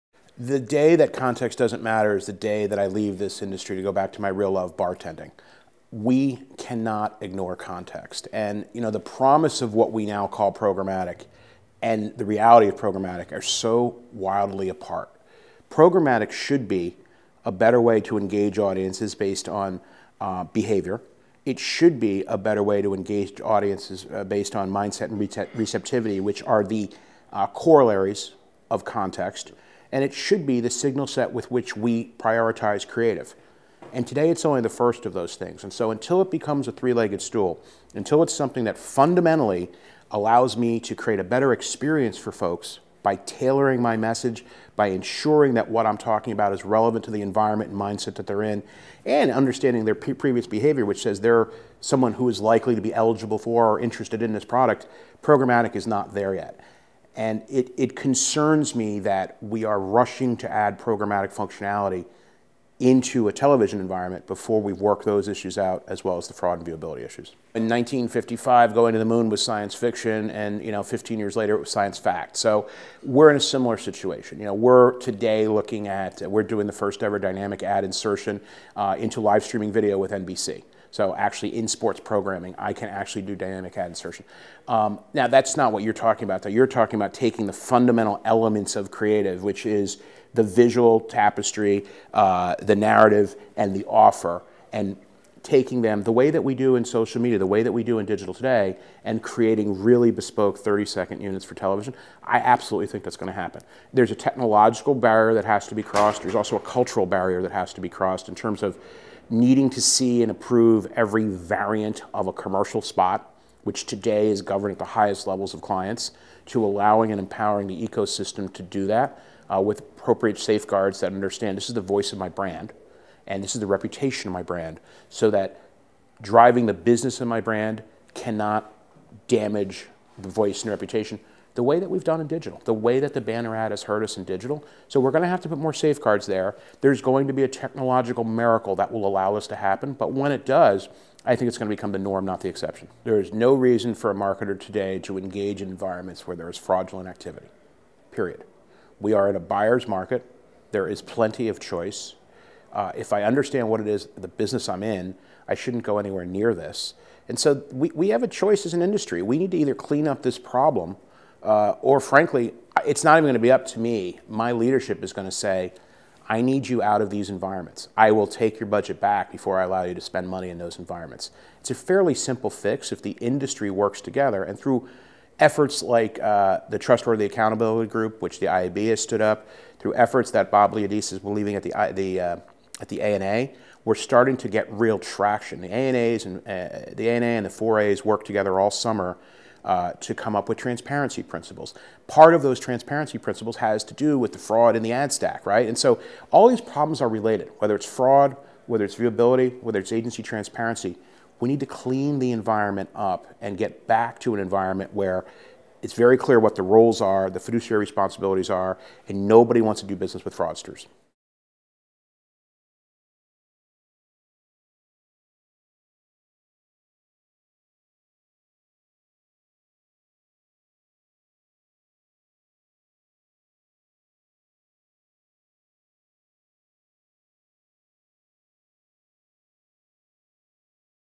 More Safeguards Needed For Dynamic Ad Creative: BoA's Paskalis
The concept of dynamic ad insertion - through which, consumers would get served a brand ad with different content, depending on their targeting profile - has been talked about for a couple of years now. The exciting reality may be around the corner, if the industry can persuade brands it is a safe one, says a major-brand marketing chief.